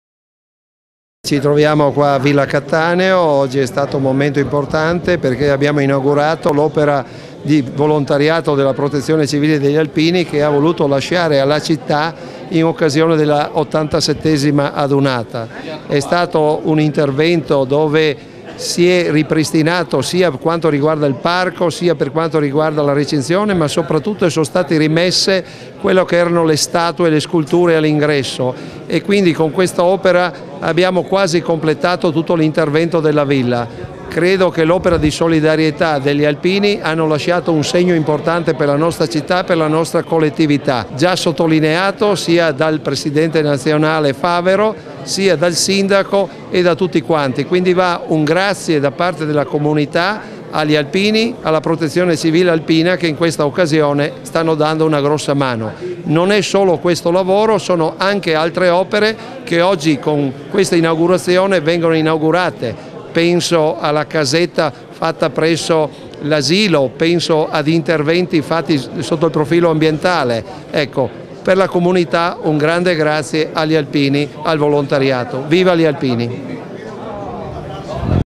Pordenone: Inaugurazione opera di Protezione Civile
Intervista a Bruno Zille, assessore comunale di Pordenone